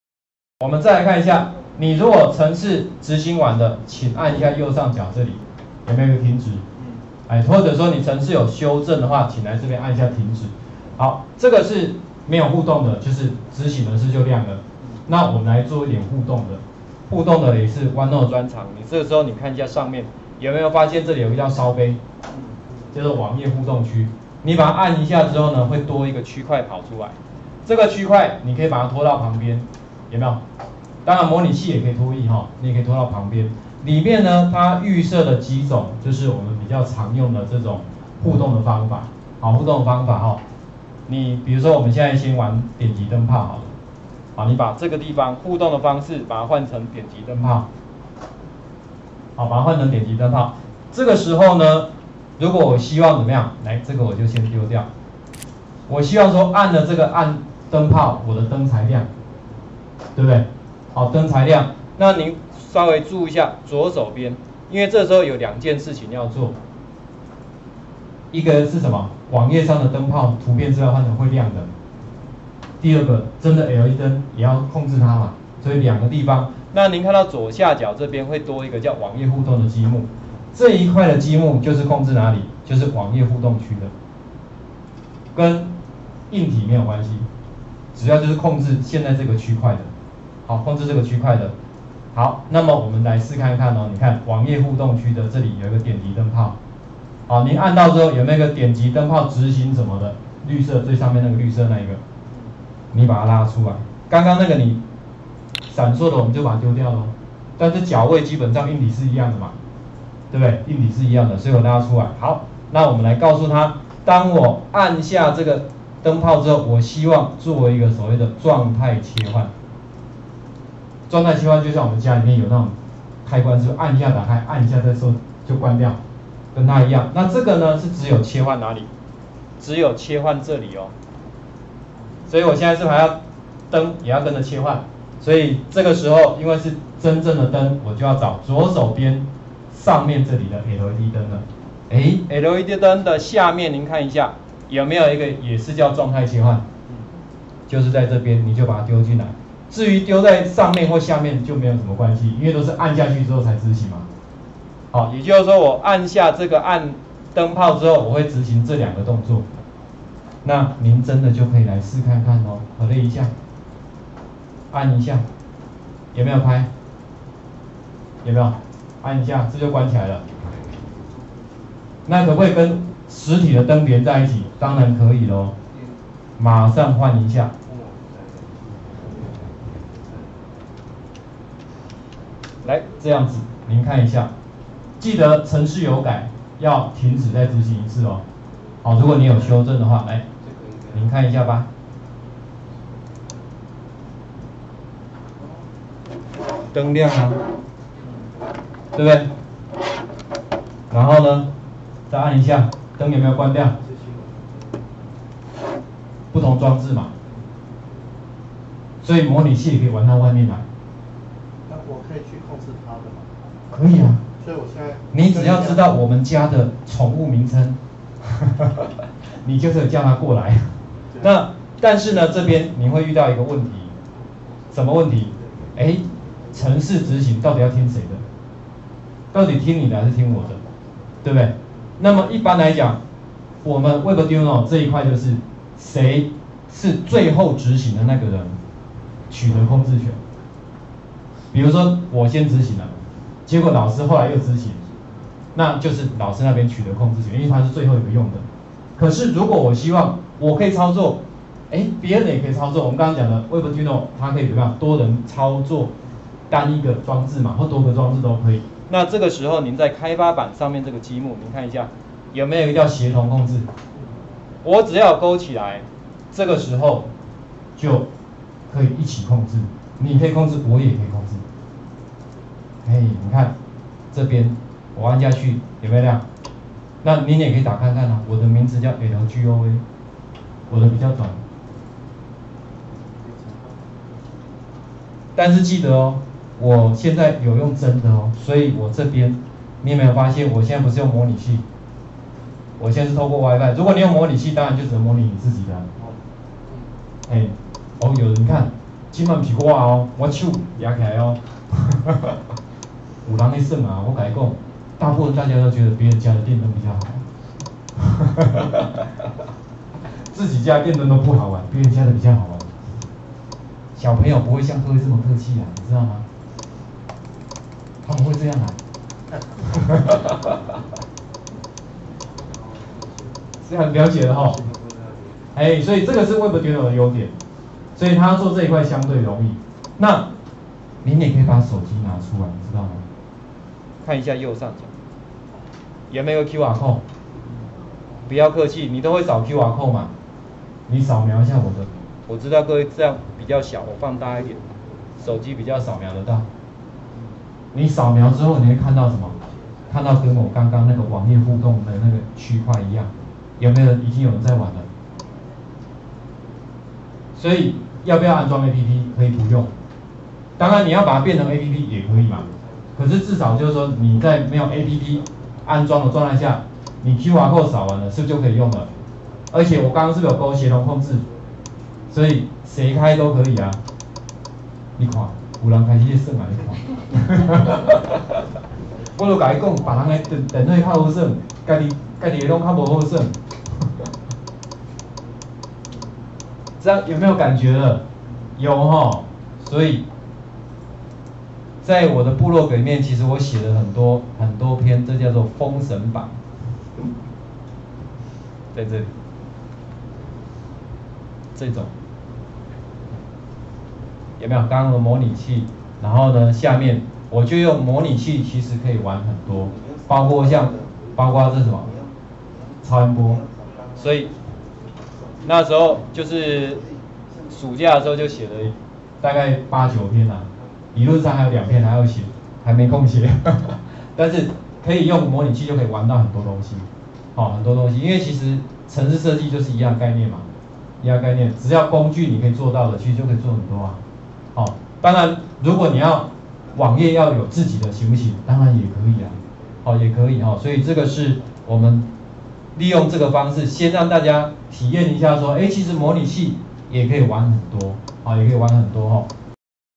06：使用模擬器與實體LED燈連結
此影片為「20171025聖約翰科大：跟著 Webduino Dr. Smart 套件一起翻轉 IoT 物聯網」研習影片！